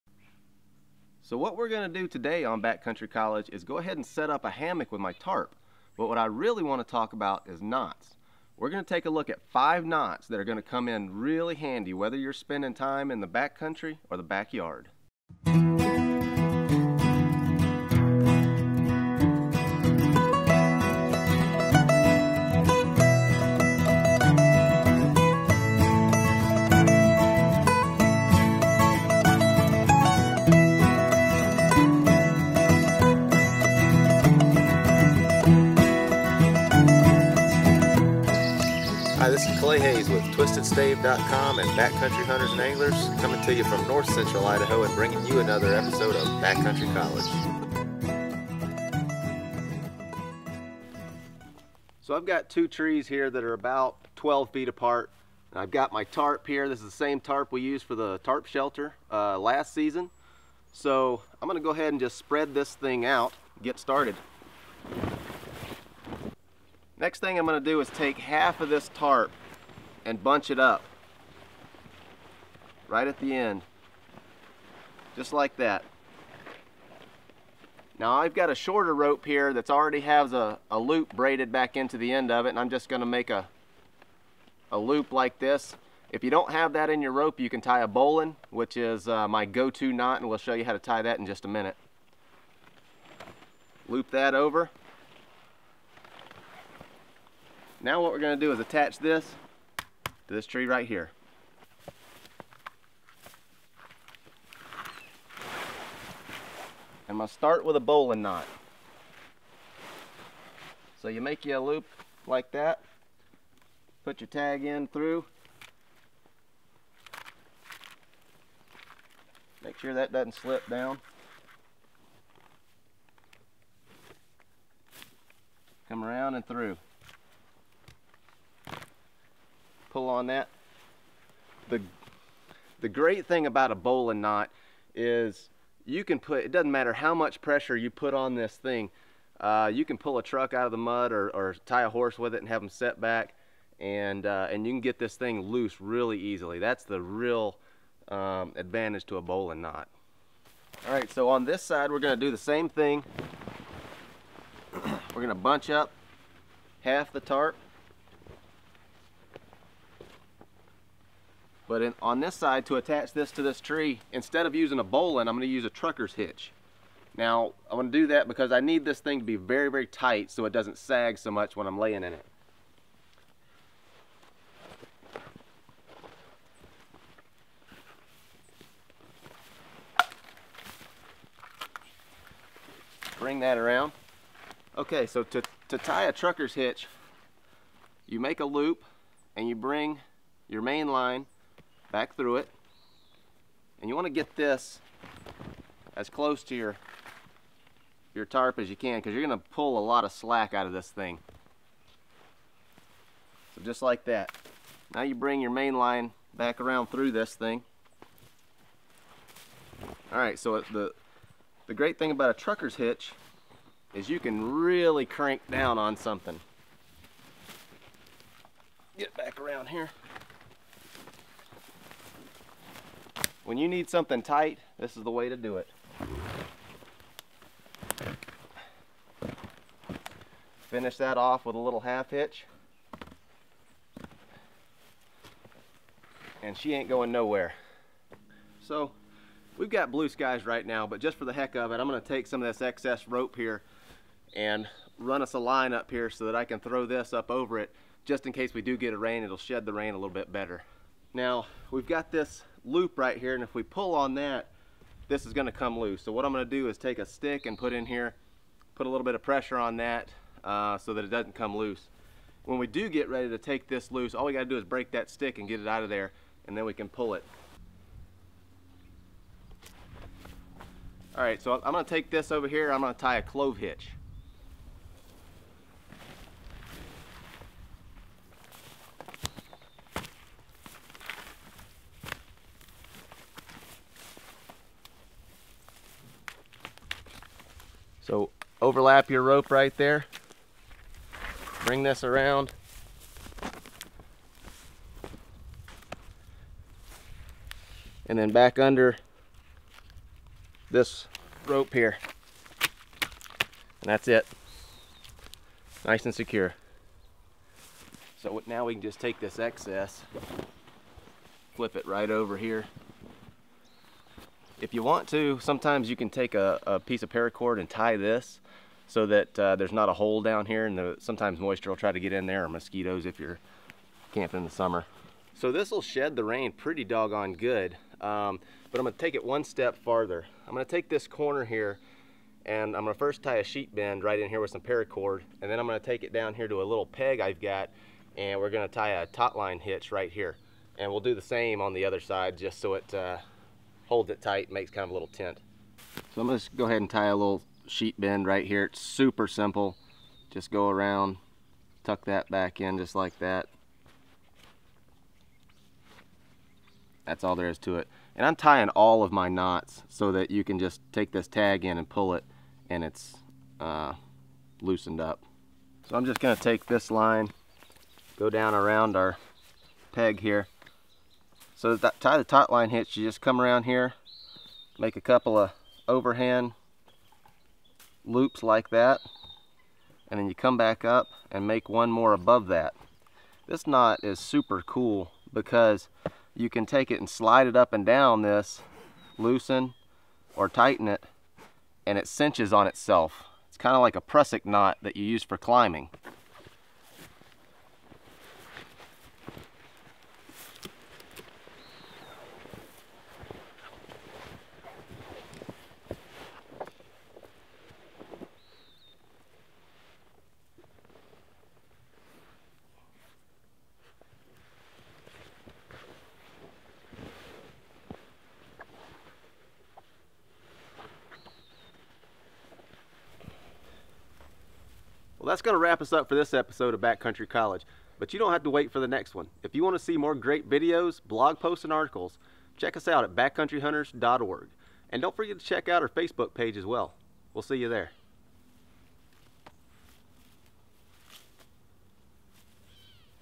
Backcountry College - knots and such
In this episode of Backcountry College, we'll set up my canvas tarp into a hammock, but the real focus will be on the different knots we'll use to do it.  We will learn to tie a bowline knot, truckers hitch, sheet bend, clove hitch, and taut line hitch.